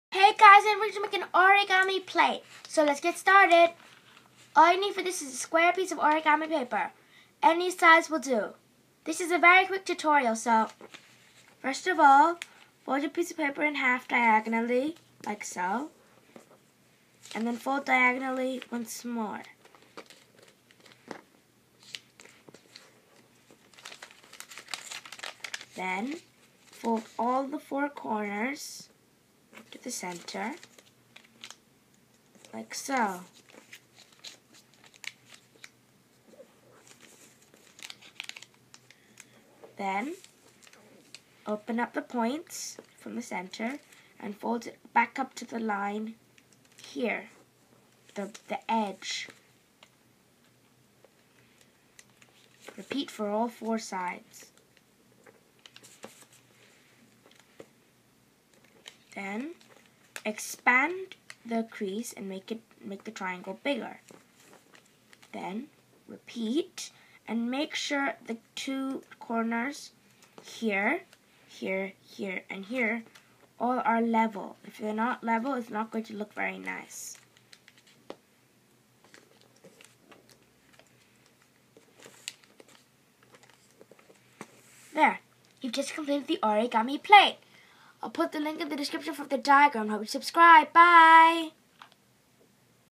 How to Make an Origami Plate
Hi Guys! So sorry it's been 10 days since I've made a video (this is my eighth)! There isn't really a diagram from this very basic plate, so I think you'll just have to copy. Bye!